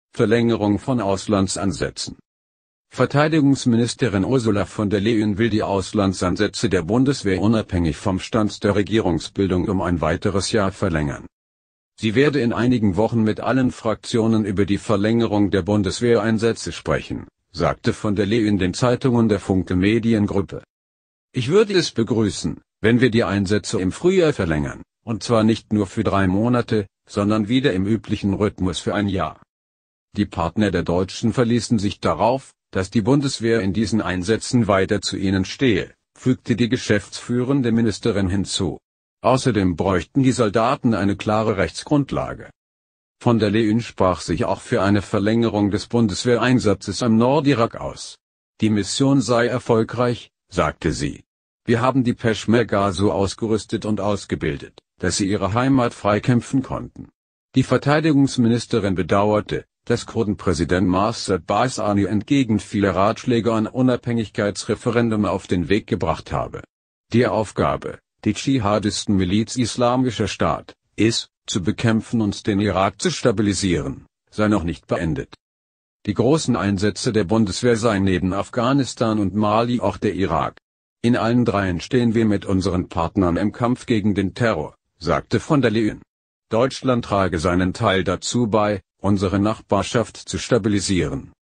Von der Leyen will Einsätze der Bundeswehr verlängern
Verlängerung von Auslandseinsätzen
Verteidigungsministerin Ursula von der Leyen will die Auslandseinsätze der Bundeswehr unabhängig vom Stand der Regierungsbildung um ein weiteres Jahr verlängern.
Sie werde in einigen Wochen mit allen Fraktionen über die Verlängerung der Bundeswehreinsätze sprechen, sagte von der Leyen den Zeitungen der Funke Mediengruppe.